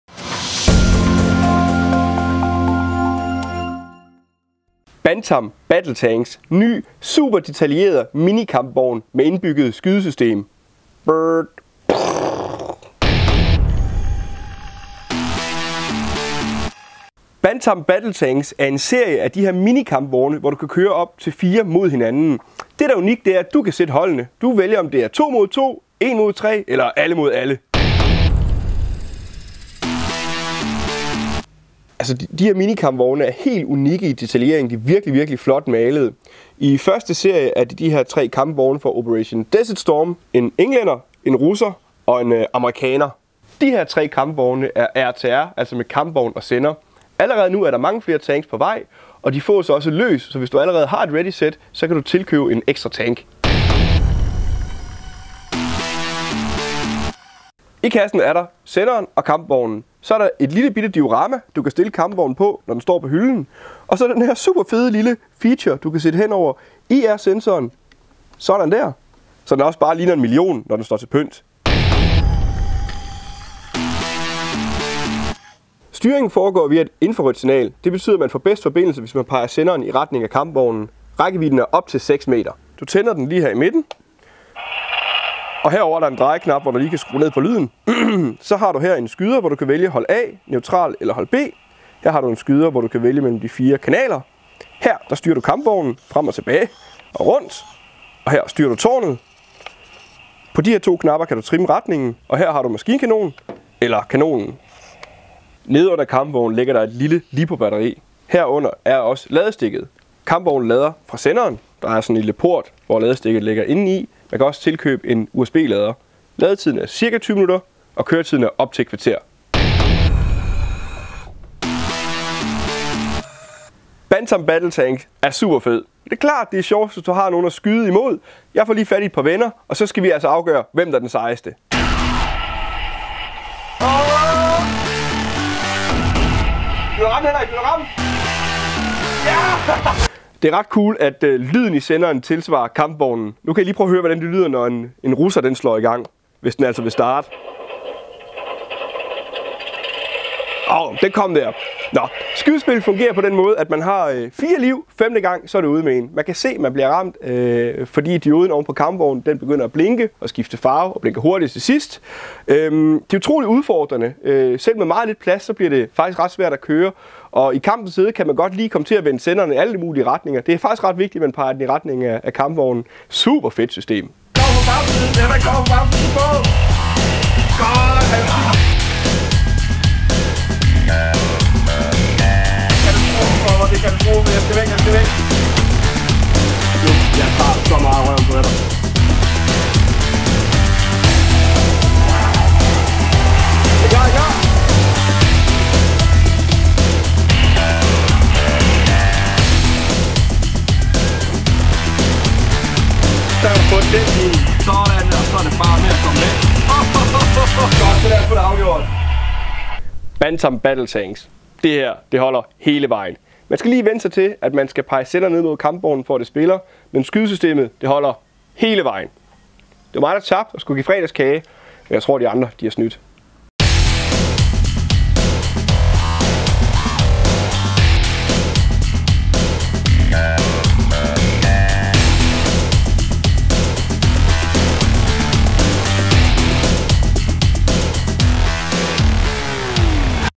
Fjernstyret kampvogn med skydesystem - Bantam Battle Tank
Bantam Battle Tank er en skala 1:72 fjernstyret kampvogn med infrarødt skydesystem. Du kan kæmpe med eller mod dine venner og se hvem der er skarpest.